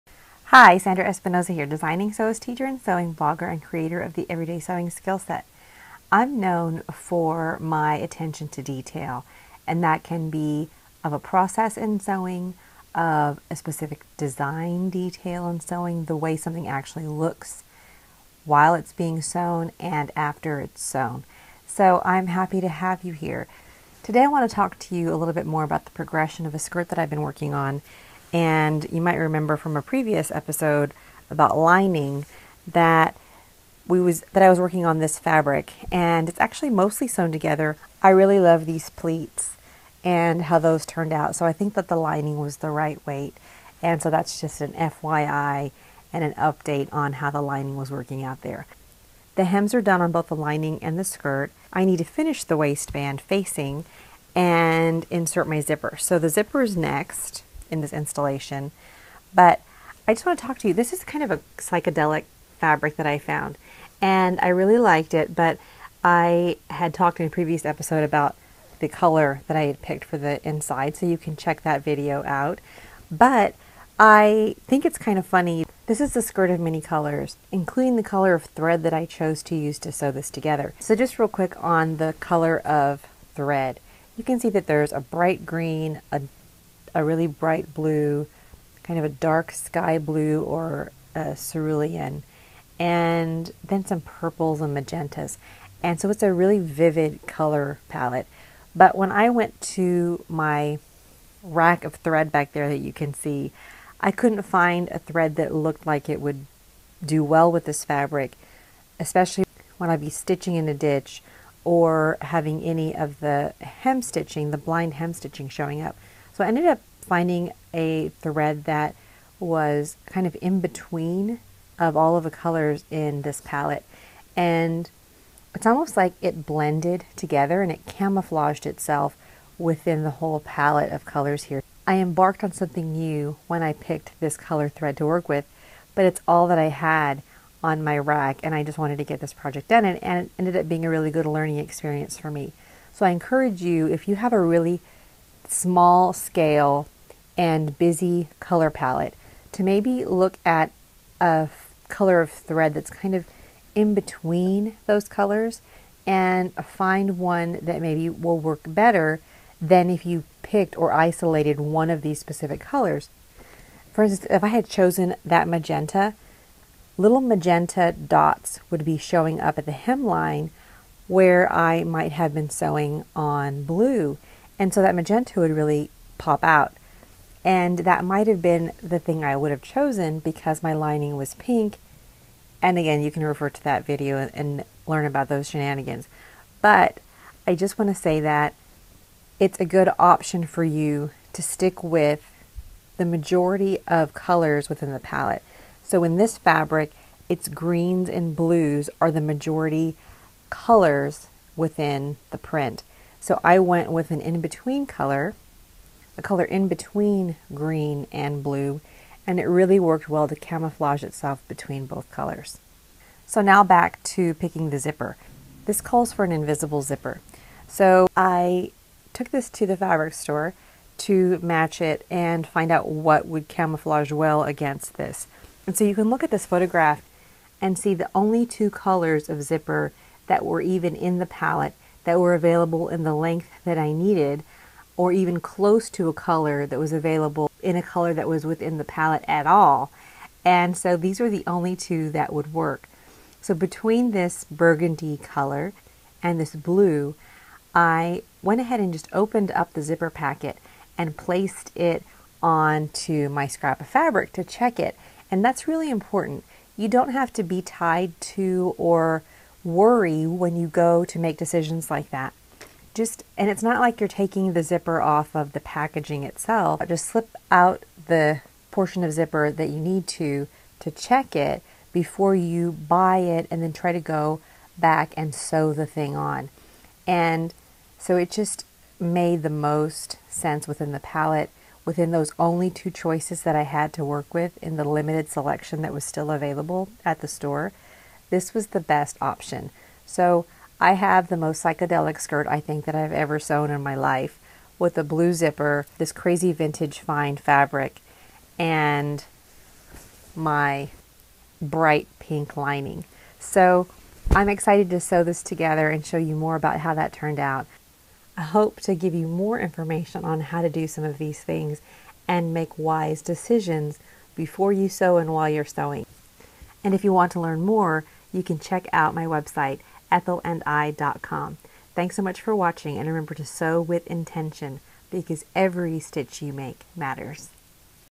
Sewing on Multi-colored Fabrics: Zippers and Threads
This video tutorial will help you make better decisions when choosing your thread color and zipper for multi-colored fabrics. It's scary to feel confused when you are buying these things and have to commit to sewing with them.

Just play with it. Have fun. Take a scrap of fabric with you when you're shopping for these things, and don't forget to unwind a bit of thread and slide the zipper out of your package!

Place it on your remnant fabric, then pick the best one. By that I mean the color that blends in or camouflages the best.